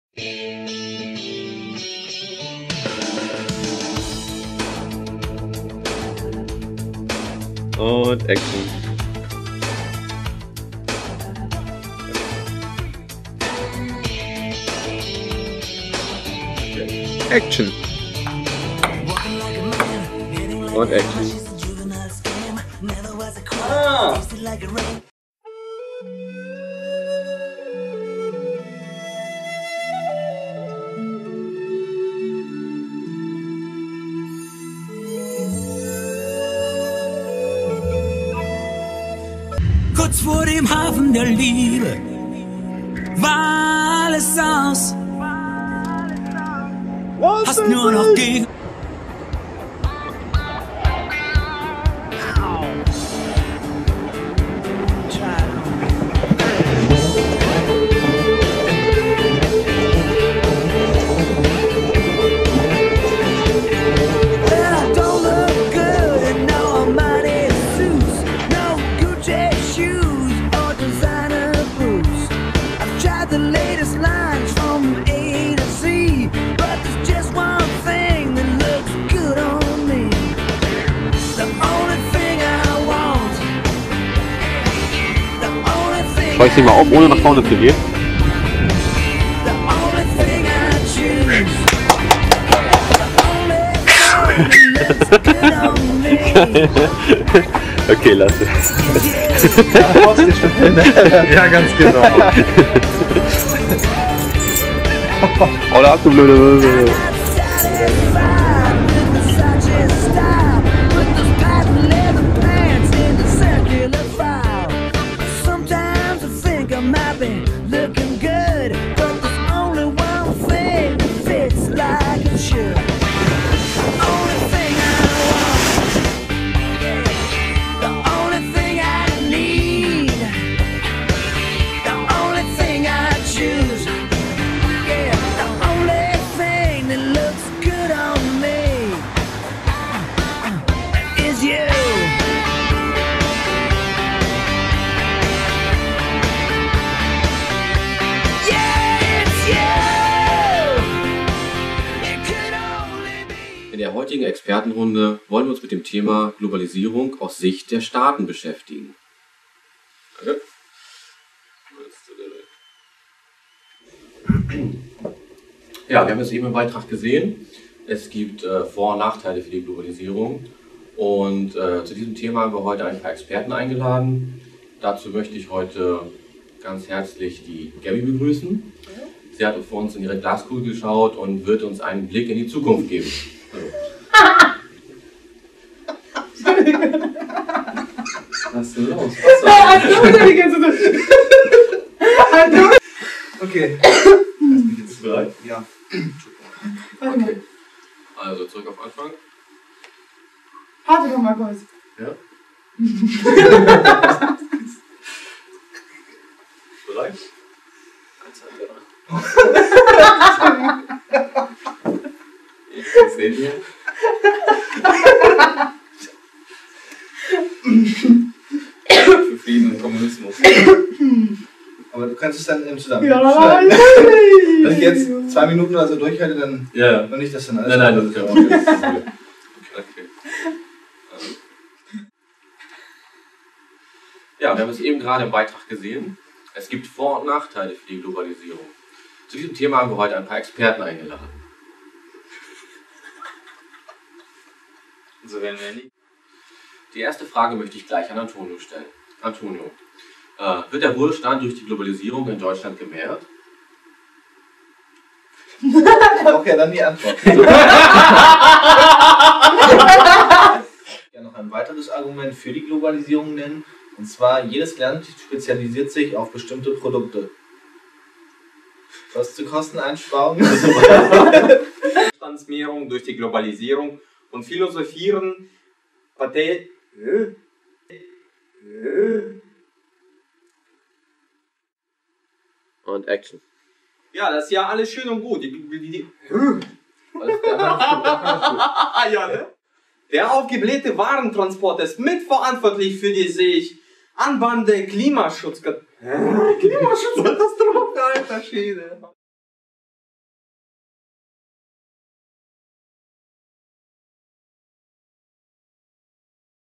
Making Of - Was ist Globalisierung von GAMA - Wissen
Videoschulbeitrag zum Thema: "Globalisierung aus Sicht von Staaten" Was ist Globalisierung von GAMA - Wissen